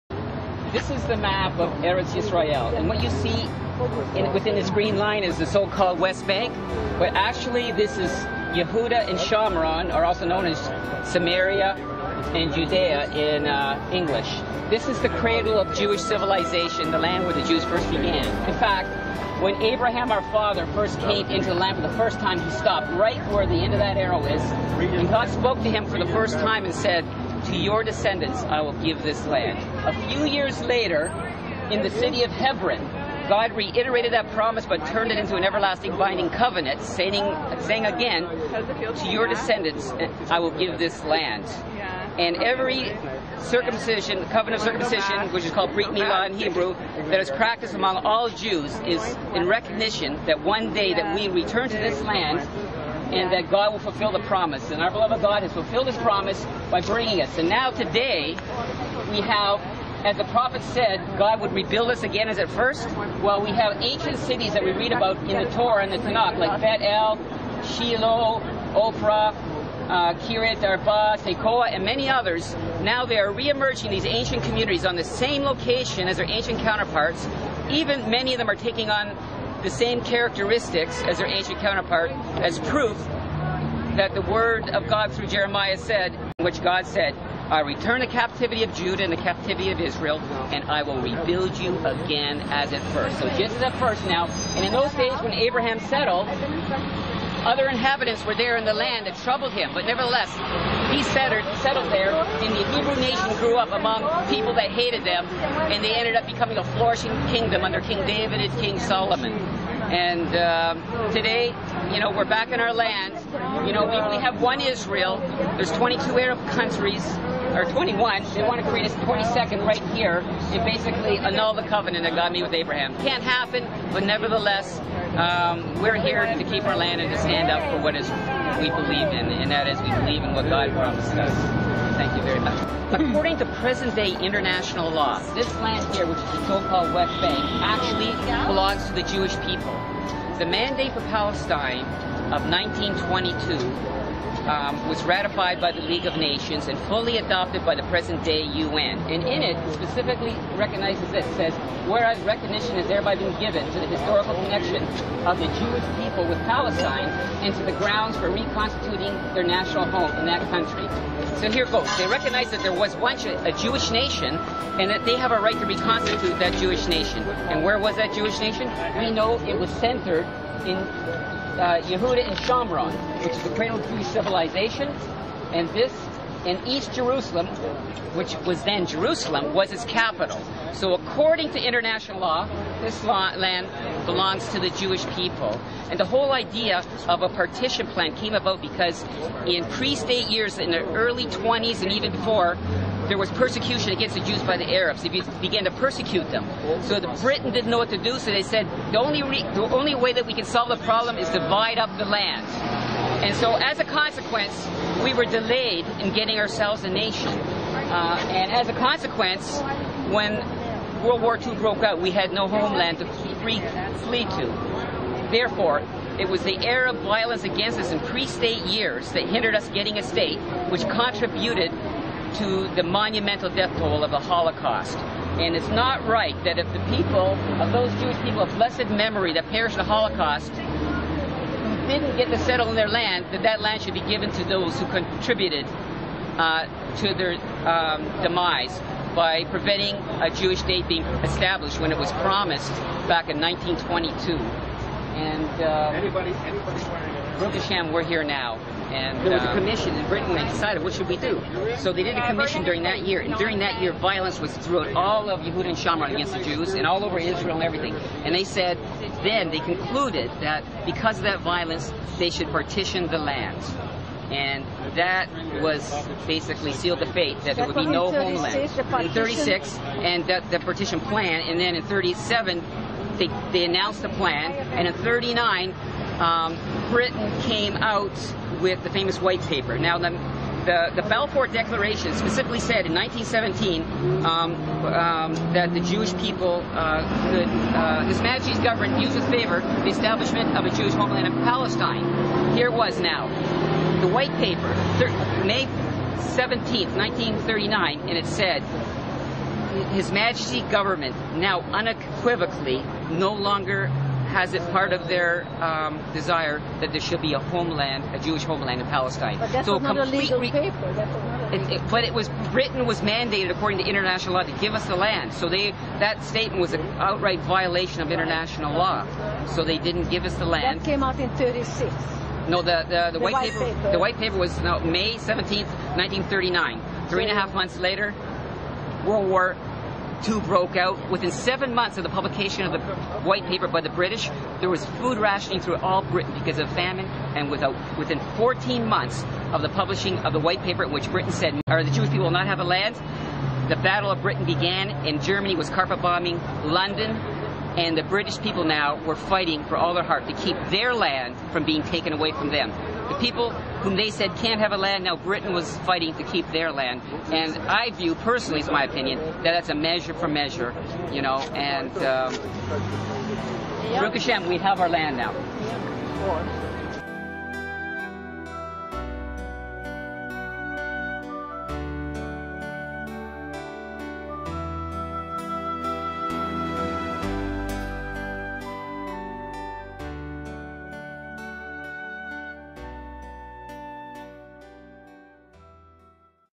Jewish Heritage.wmv
B"H Ezra tells the world that legally the so called west bank land belongs to the Jewish people. According to the Mandate for Palestine of 1922, this land known to us as Yehuda and Shomron or Judea and Samaria, was given to us.This is the land at the cradle of Jewish civilization. This is Our Beloved land and was given to us by Our Beloved Father G-d.  Ezra B Ridgley See documentary, "The Spring of Judea and Samaria'